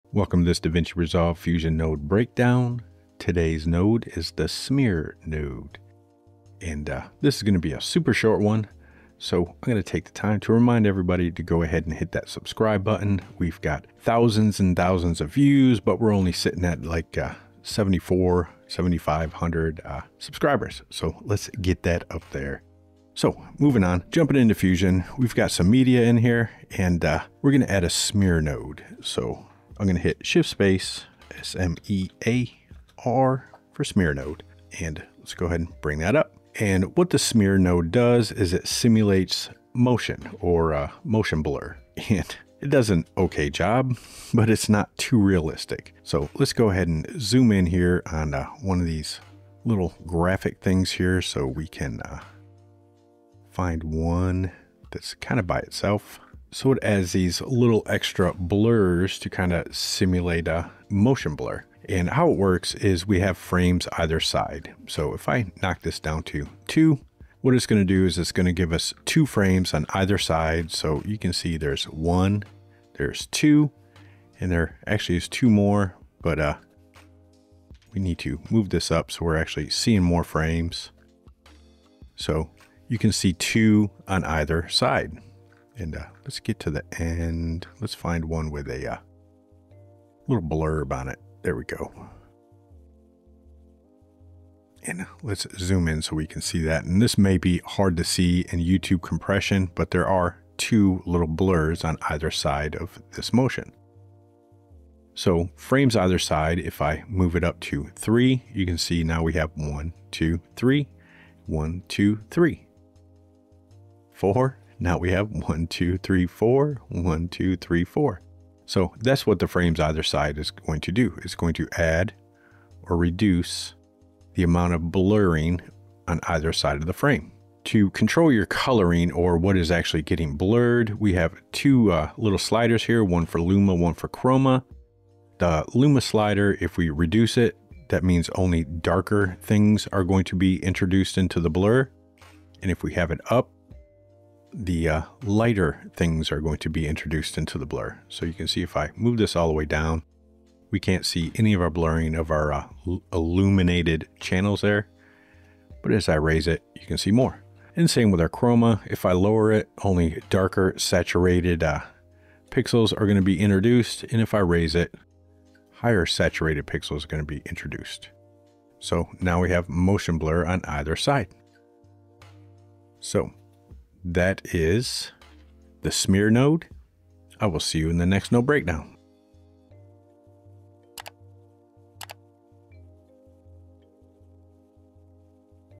DaVinci Resolve 20 Fusion Smear Node
Daily node breakdown of Resolve FX nodes used with DaVinci Resolve 20 Fusion as OFX nodes. In today's node we cover the Resolve FX Smear Node.